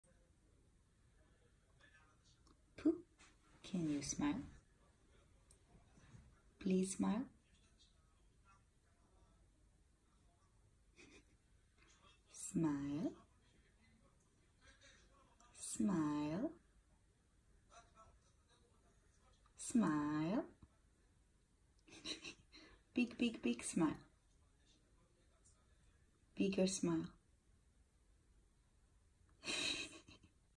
Tiny dog will make your day with this adorable smile!
Pooh is a little sweet pomaranian. And when i ask this Good boi to smile he lets out biggg beautiful adorable smile!!
This will absolutely make your day